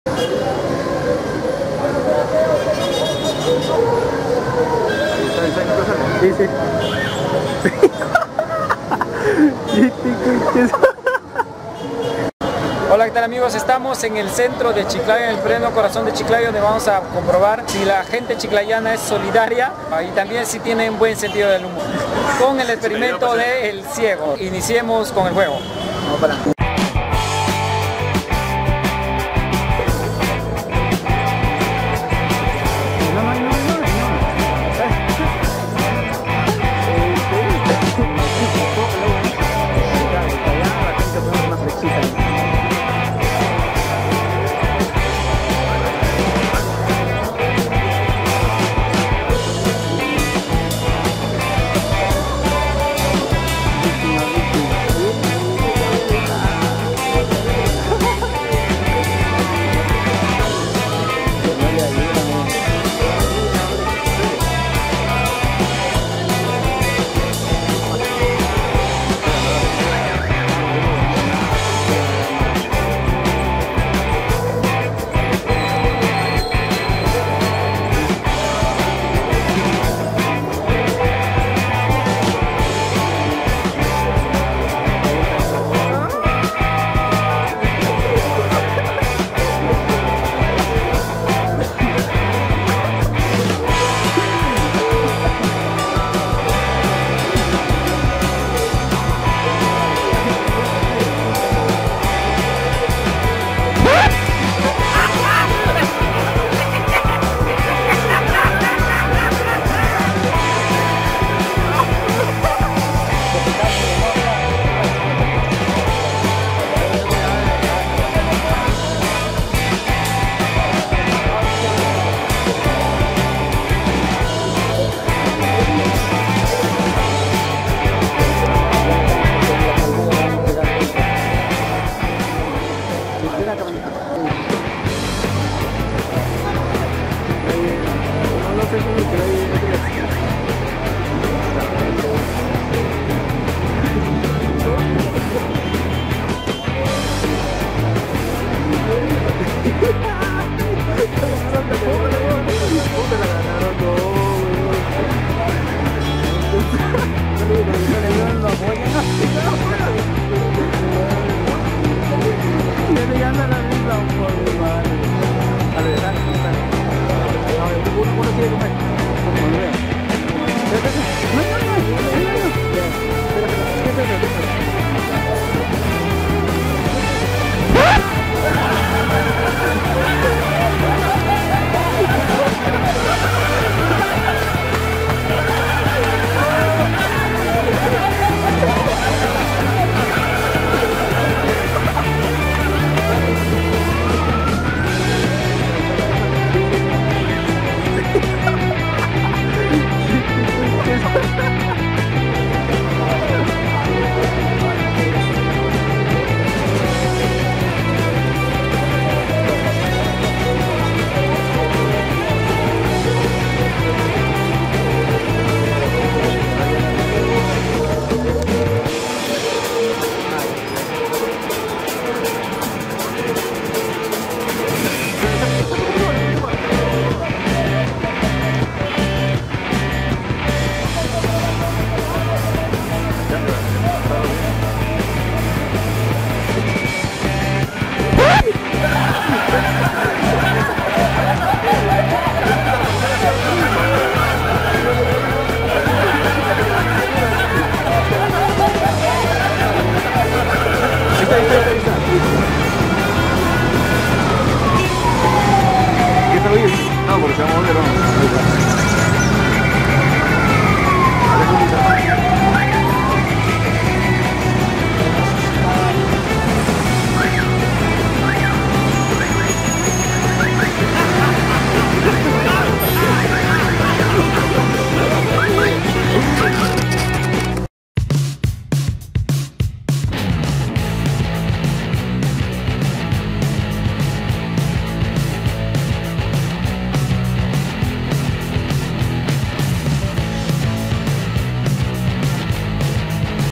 FELIZ DÍA DE LOS INOCENTES - BROMA DEL CIEGO - CÁMARAS ESCONDIDAS
CÁMARAS ESCONDIDAS CHICLAYO PERÚ - BROMA EL CIEGO
esperamos les divierta si es así ya saben den "me gusta" comenten y compartan con todo sus amigos, nos vemos en el próximo vídeo.